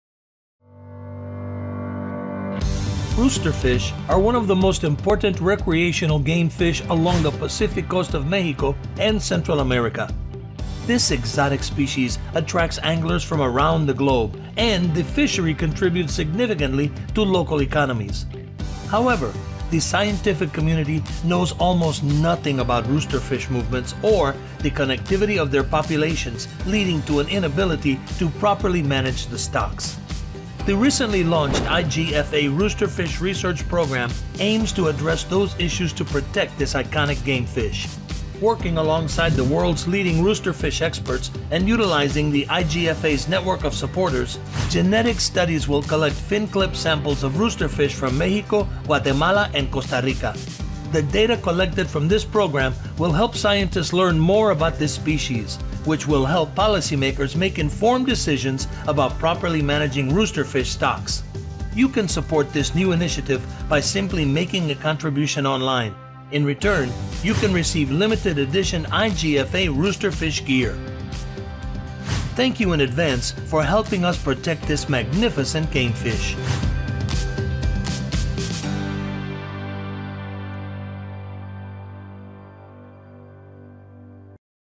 IGFA Roosterfish Research Program
The Roosterfish Research Program needs to raise $100,000 to conduct genetic testing over the two-year span of the study.

You can help us learn more about roosterfish to better understand information about the species necessary for proper management and conservation.